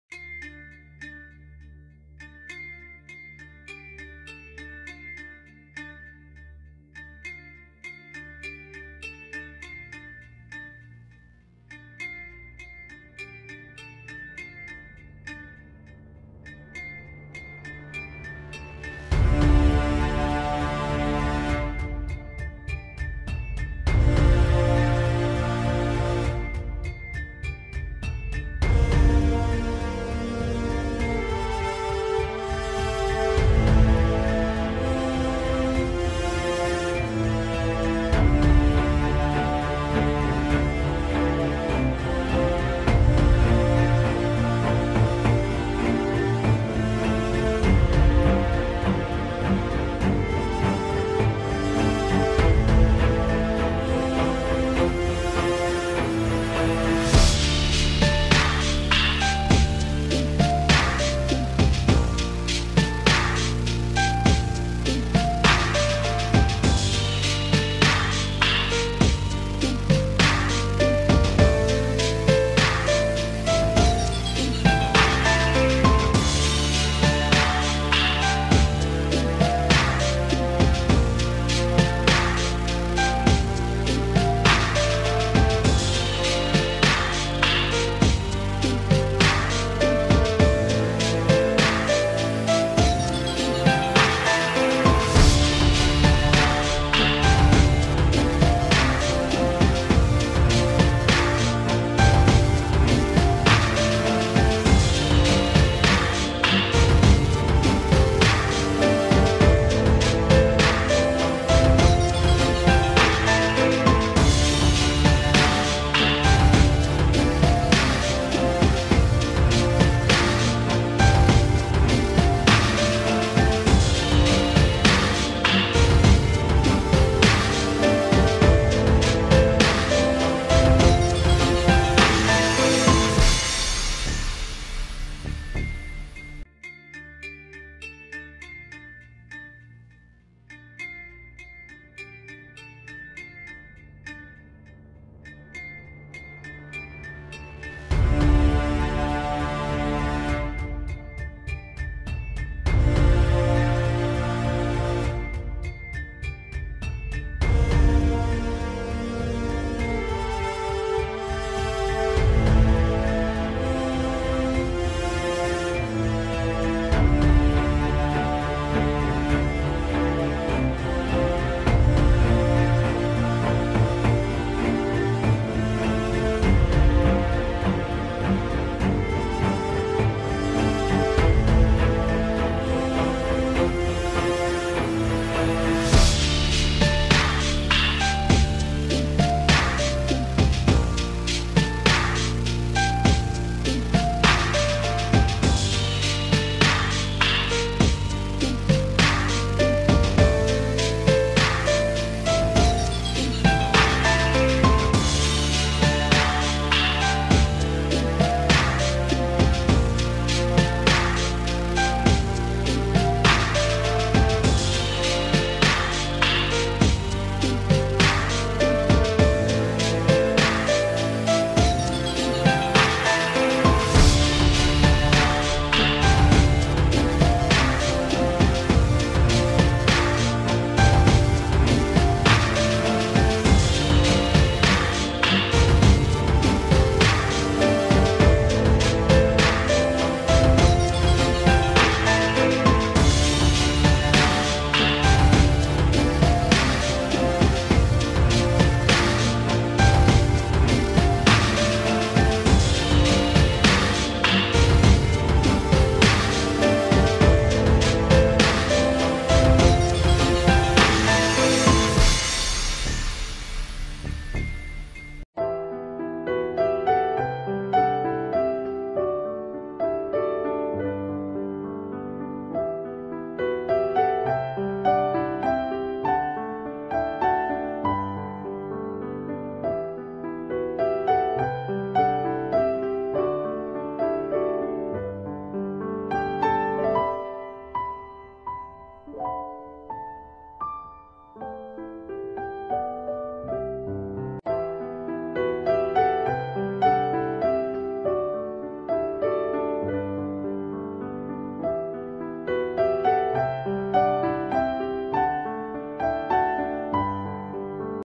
writing alphabet letters/ education vedio
Writing English alphabet letters
4 years girl